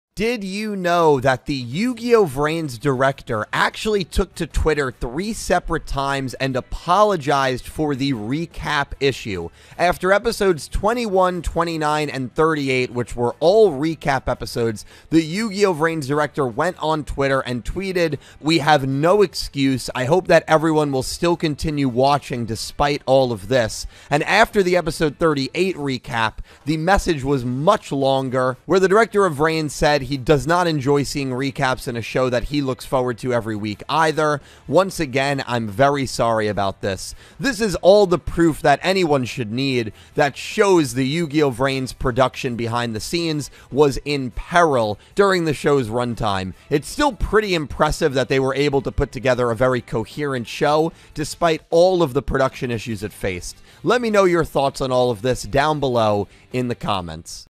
VRAINS Director APOLOGIZED for Recaps - Yu-Gi-Oh Did You Know (#48)
This was a pretty scary and crazy time if you were watching Yugioh live. 4 recaps in 25 episodes, it got so bad that the Director of VRAINS actually took to Twitter MULTIPLE times to apologize for the recaps that plagued the first half of VRAINS.